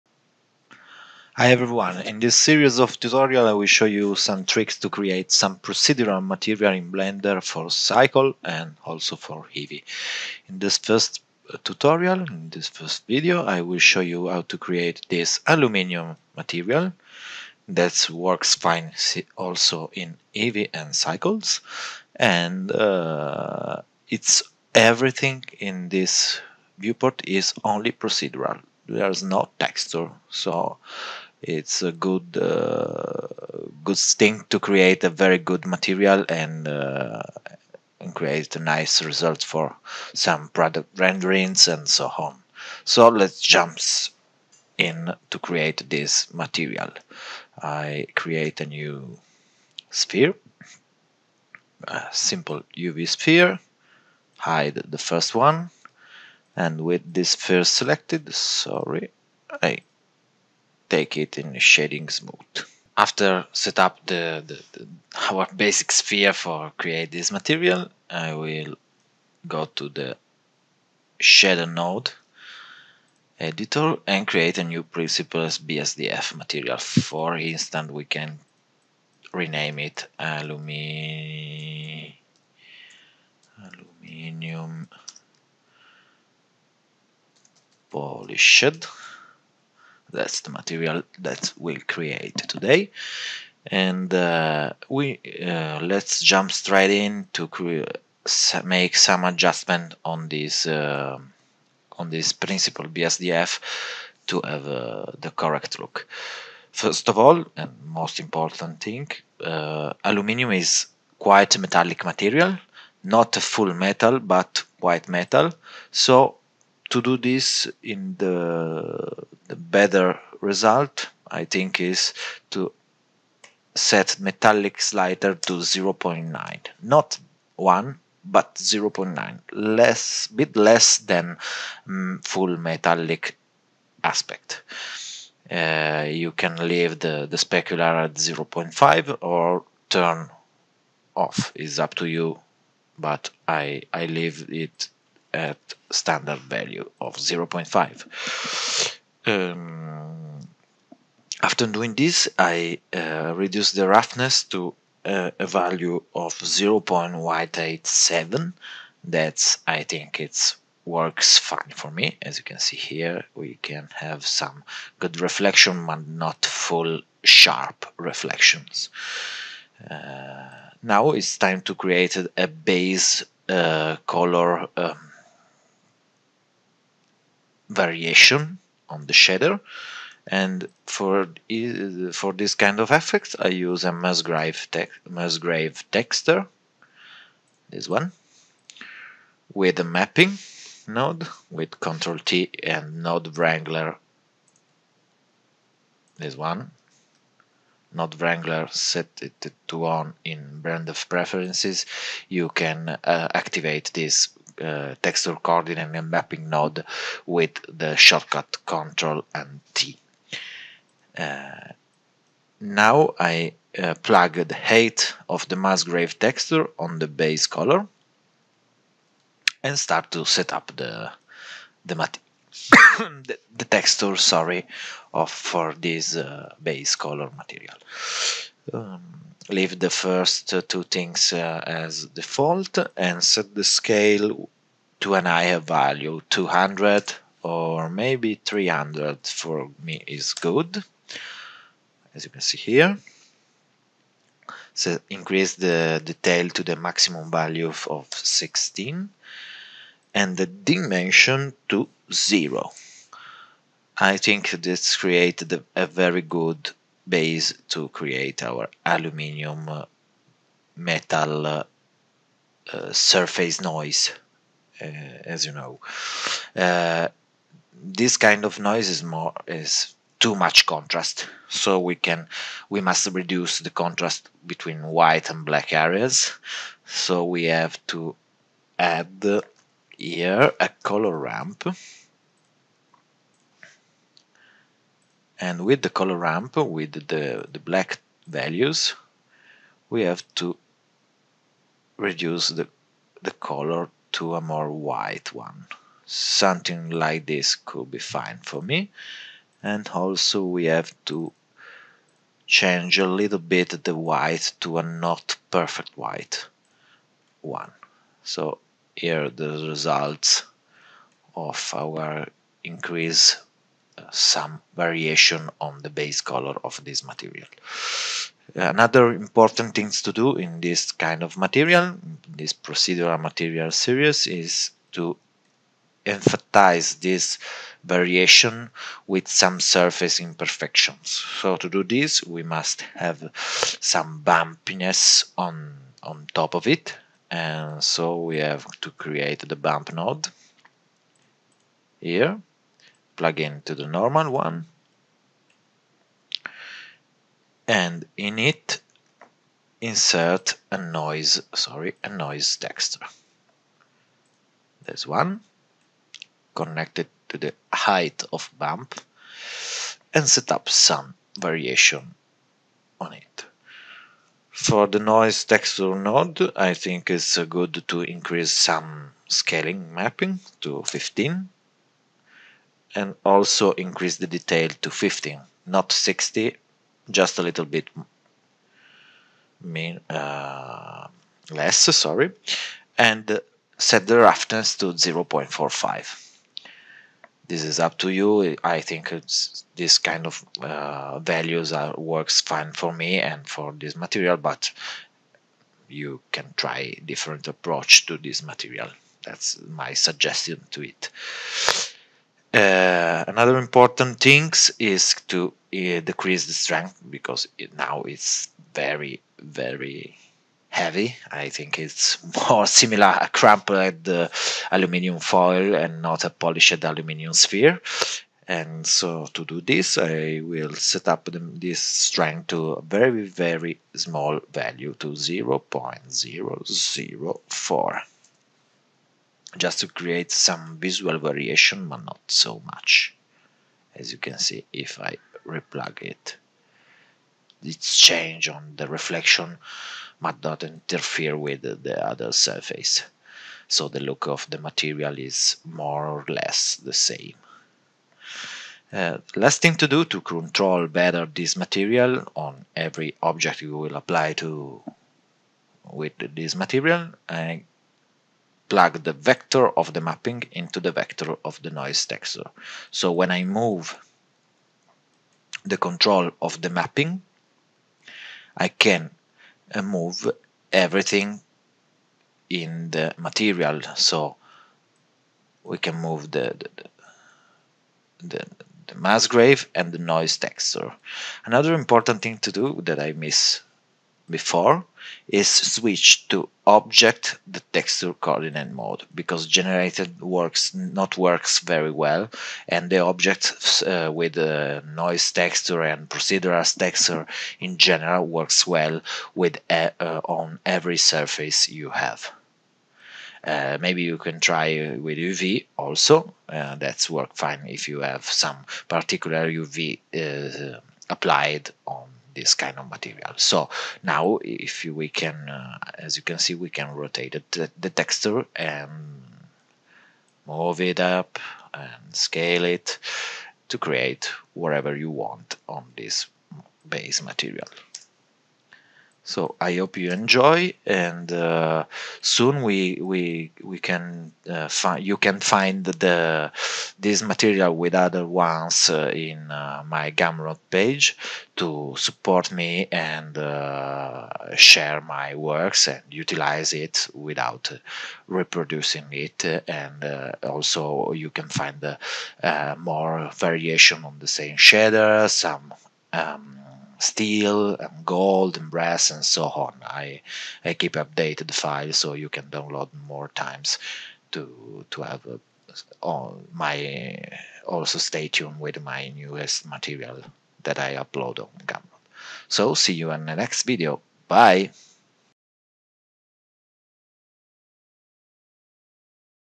Blender Aluminum Shader / Material. Full Tutorial Free + Mats on Gumroad
In this Tutorial you can see how to setup an useful aluminum shader for both cycles & eevee renderings.
If you like this videos you can find the complete material on gumroad.

Software used in the tutorial:
Blender 3.2